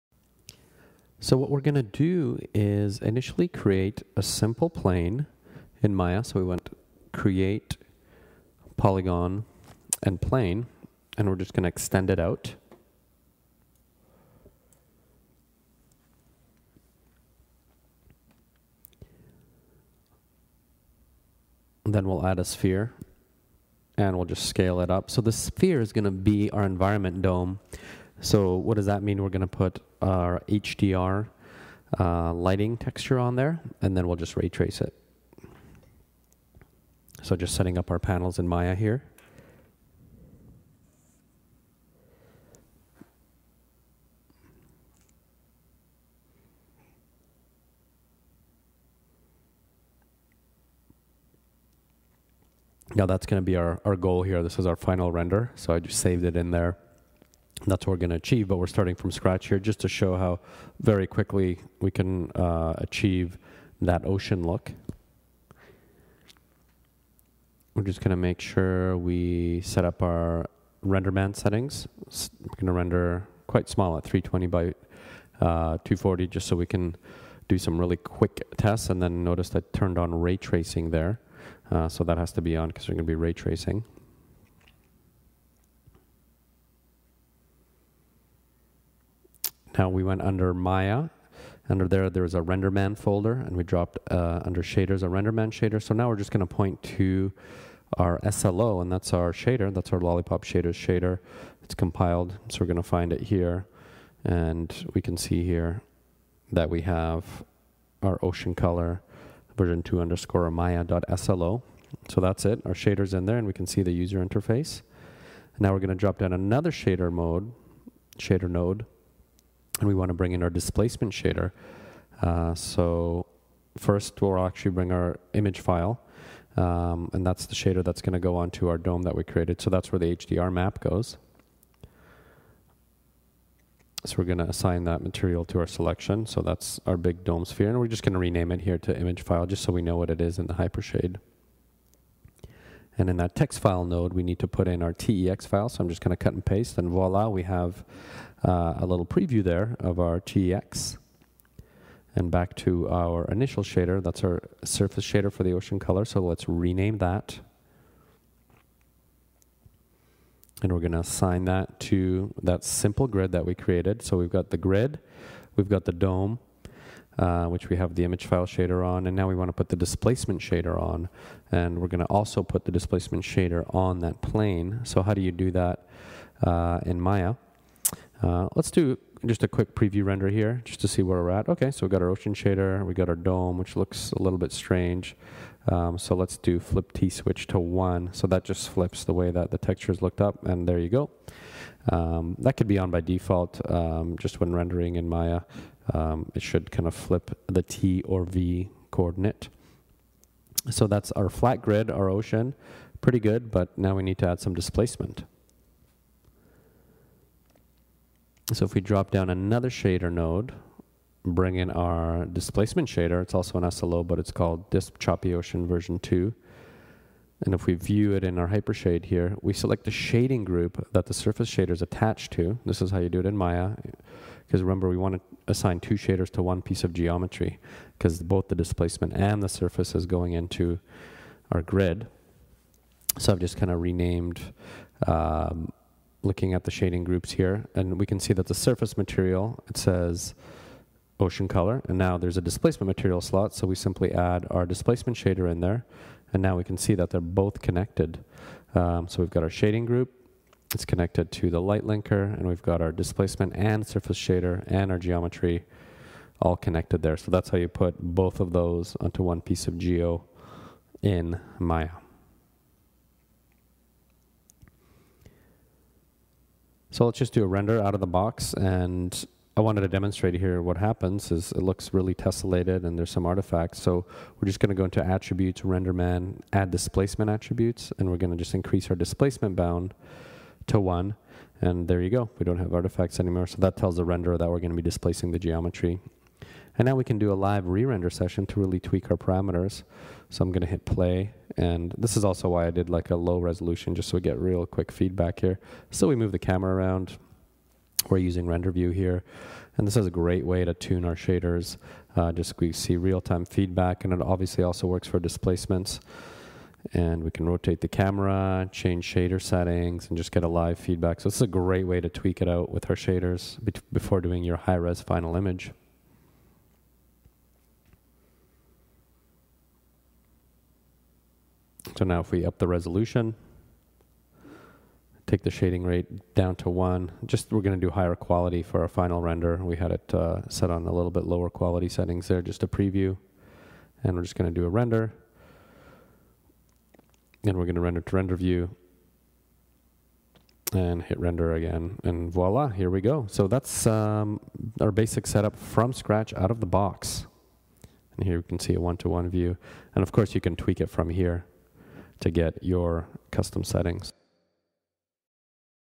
RenderMan Ocean Shader Tutorial in Maya - Part 1
Watch this 6-minute video on how to bring our Surface and Displacement RenderMan Ocean Shaders into Maya/RenderMan Studio!

This Part 1 tutorial takes you from scratch in a fresh Maya scene through all the steps to get our shaders setup in RenderMan and rendering a basic ocean. See how our settings work right out of the box!

Part 2 goes over each parameter in the displacement shader to show how to tune displacement wave settings, ocean colors, whitecaps, etc...

Part 3 goes over each parameter in the surface shader.